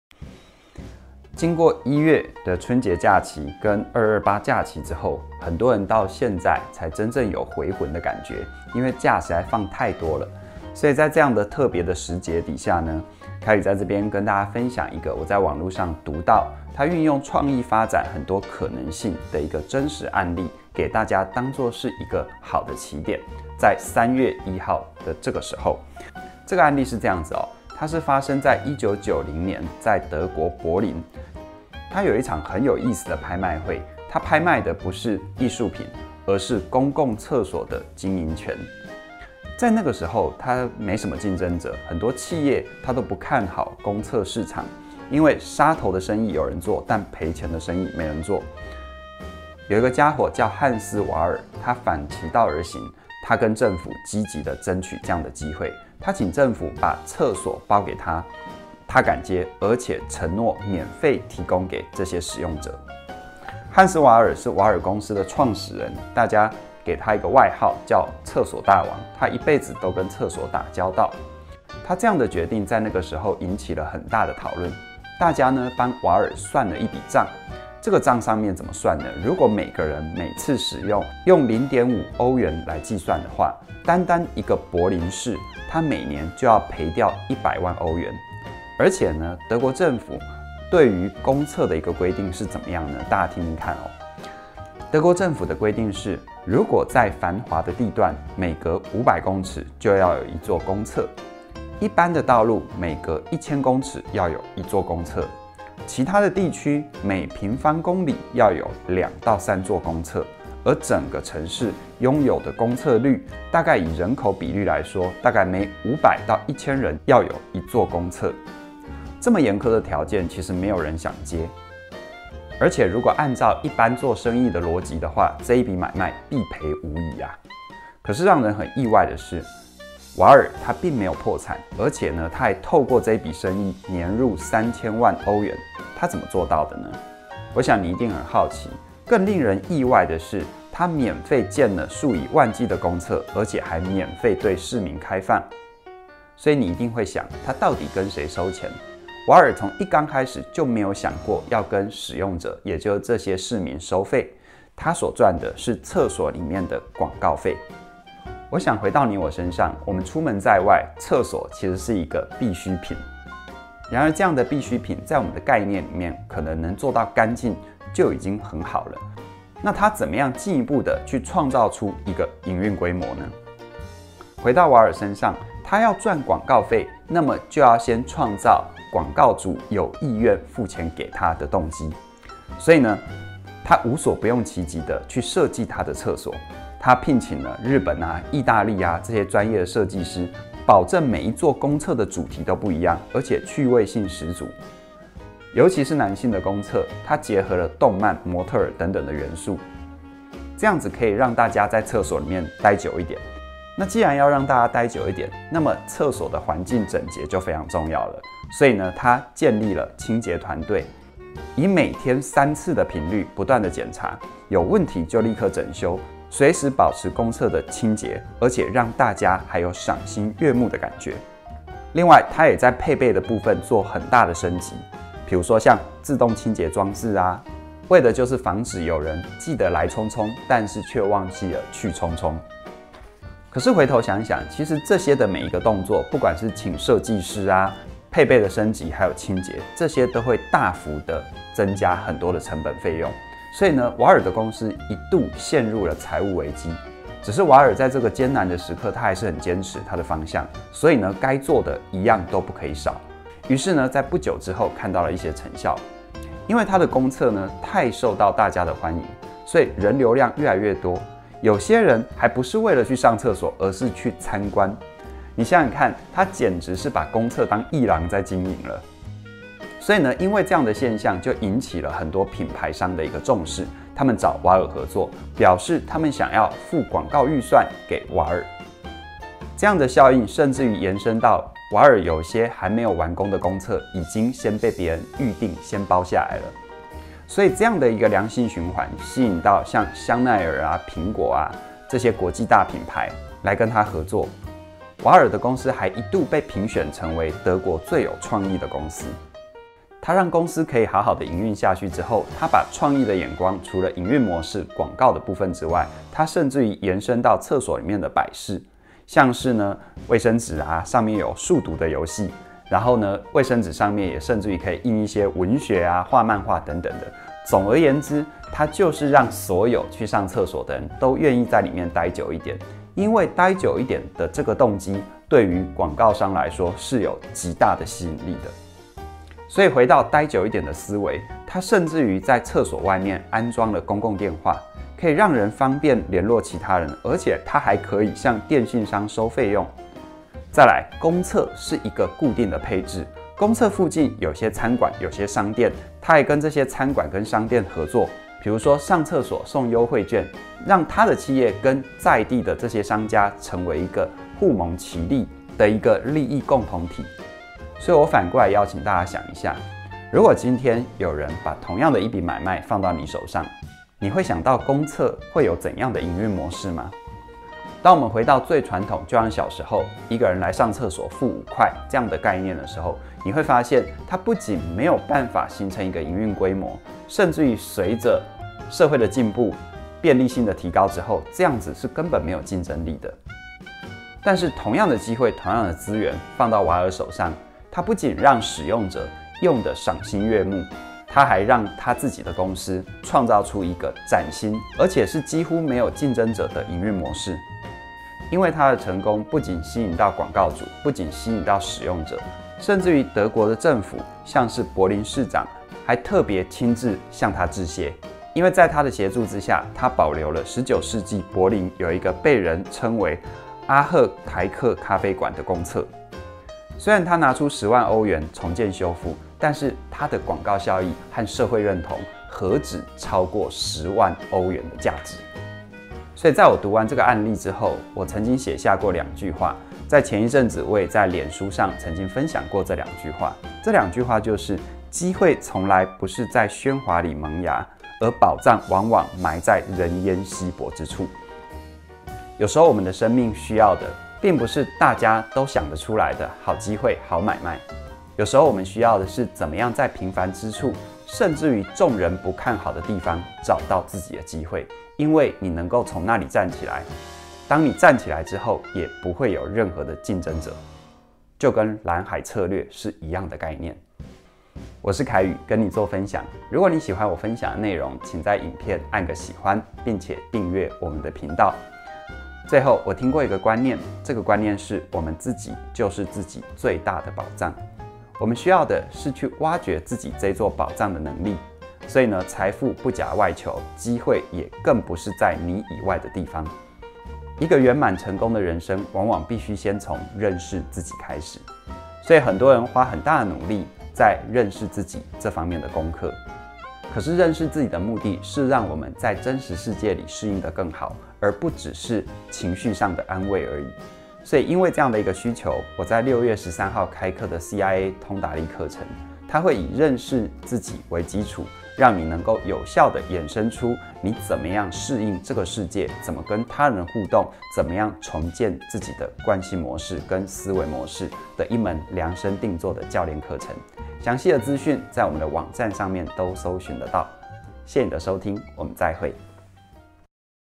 【一天聽一點#46】其實，公廁是門好生意～
如果全台灣的公共廁所給你經營，你會想出怎樣的營運模式？

使用者付費？
如果是這樣的思維，並且發生在1990年德國的柏林，那麼幾乎註定你每年要賠100萬歐元！
你還會考慮這筆生意嗎？

有個傢伙不僅接下這門生意，還讓他年入3000萬歐元。
他怎麼做到的？用怎樣的營運模式？
他的思維跟你我有什麼差異？

機會從來不是在喧嘩裡萌芽，寶藏往往埋在人煙稀薄之處。
這段分享會告訴你，寶藏埋在哪裡～

【06.13 開班】《CIA通達力》初階班~全方位溝通表達訓練(第十八期)